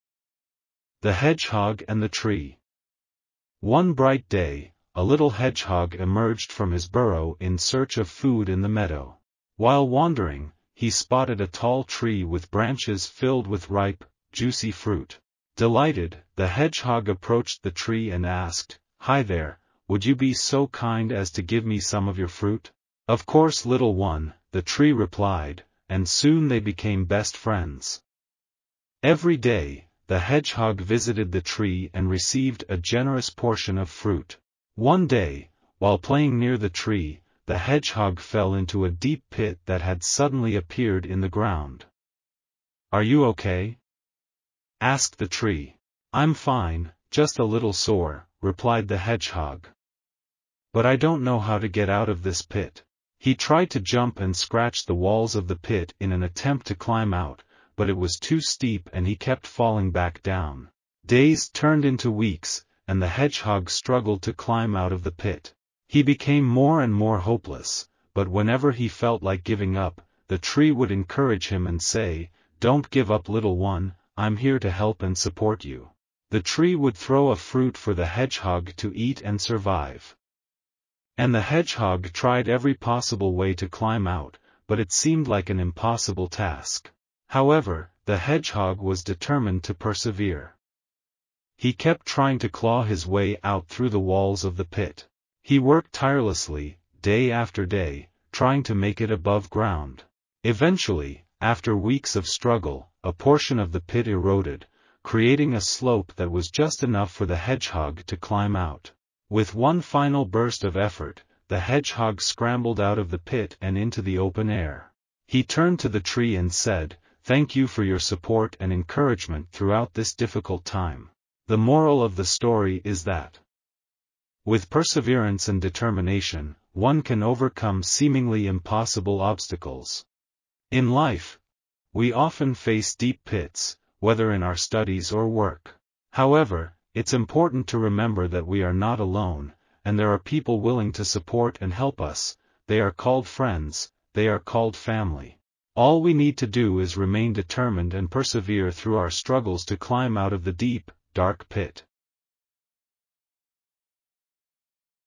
Short Moral Stories - The Hedgehog and the Tree (The Power of Perseverance) - Lessons to Learn
Short Moral and Bedtimes Stories for you and for your children.

This is a story about determination and perseverance of a hedgehog.

Popular Stories
Bedtime Stories
Moral Stories
Perseverance
Worth Reading
Motivational
Adventure
Children stories
Exciting
Interesting Stories
Fable
Family